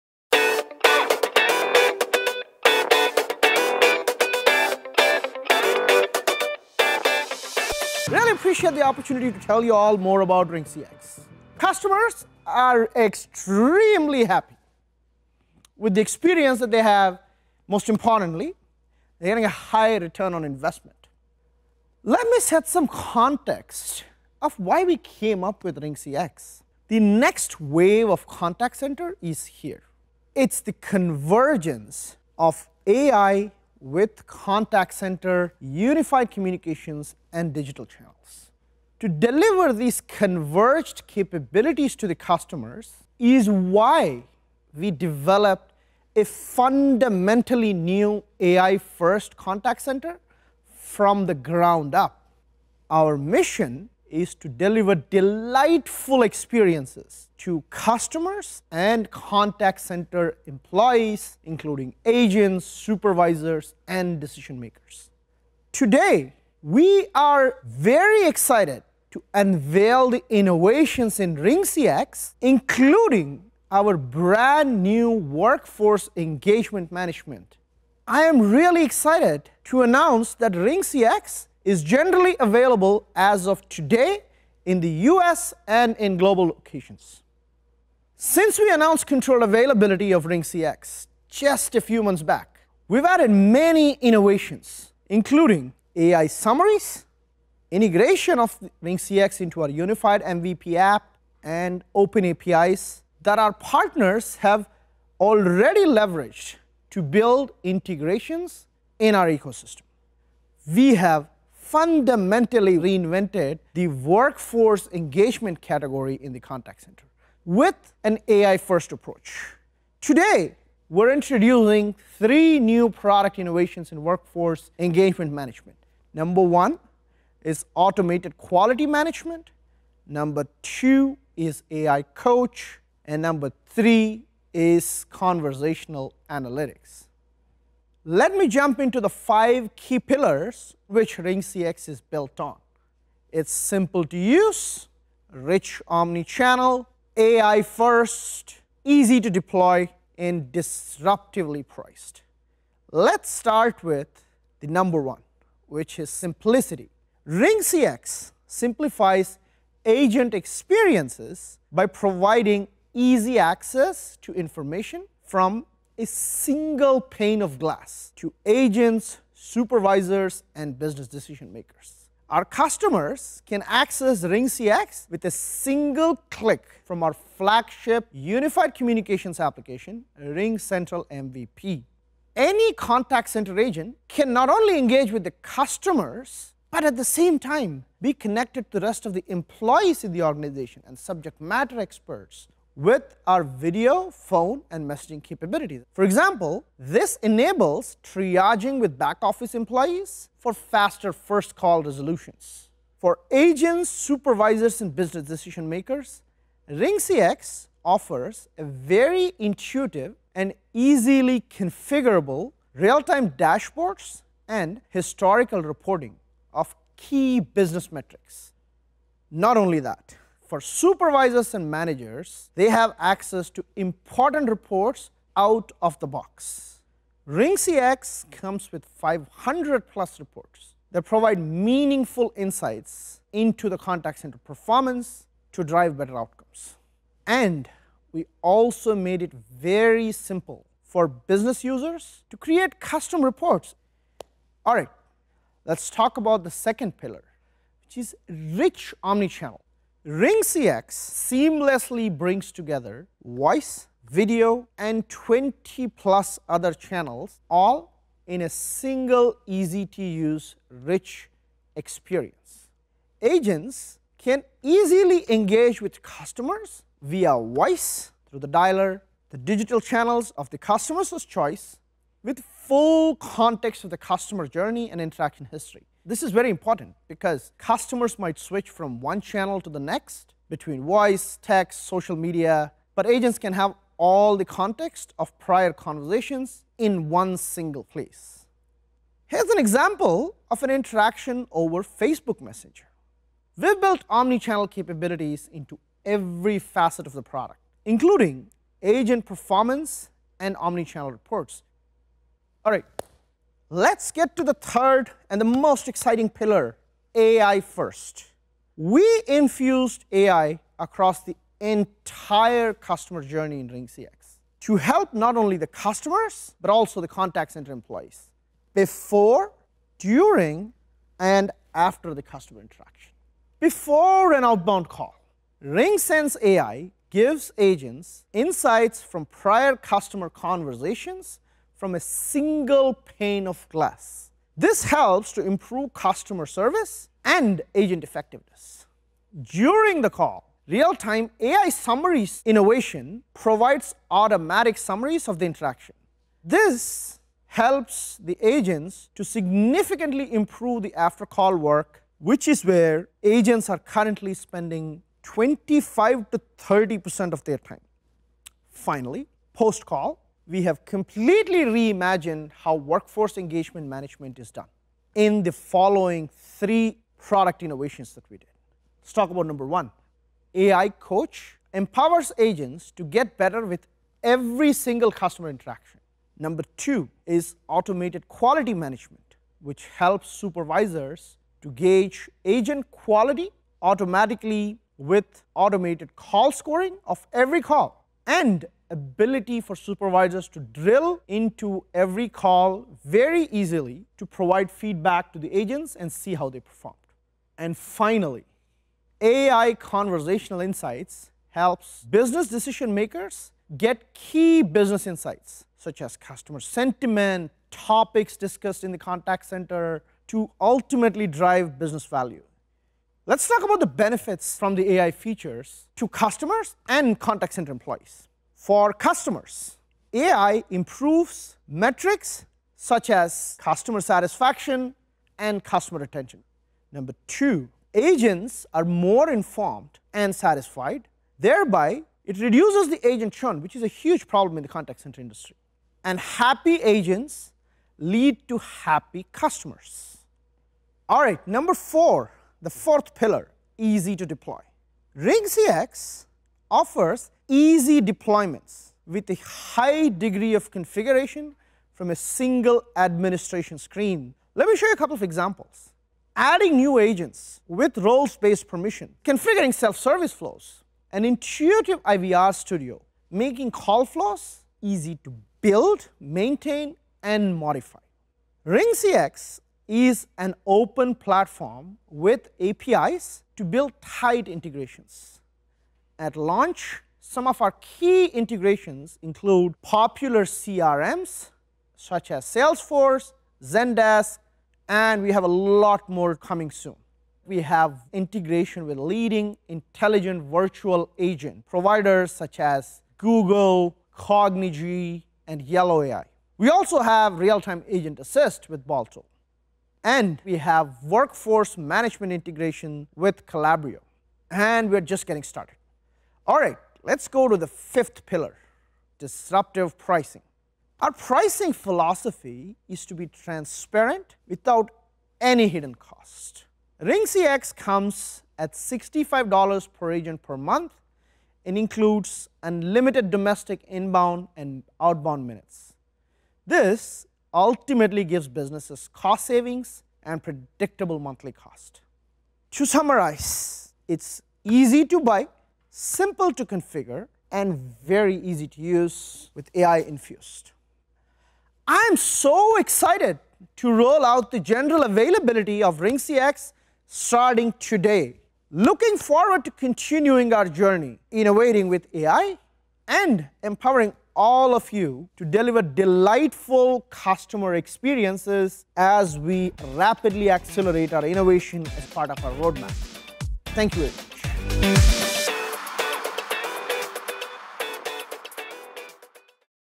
RingCX | AI-powered contact center demo
Srini Raghavan, RingCentral's Chief Product Officer, breaks down everything you need to know about RingCX, its AI-powered omnichannel contact center.

You will learn helpful tools and features of this contact center solution that will help your teams manage conversations on multiple channels, coach agents, and so much more.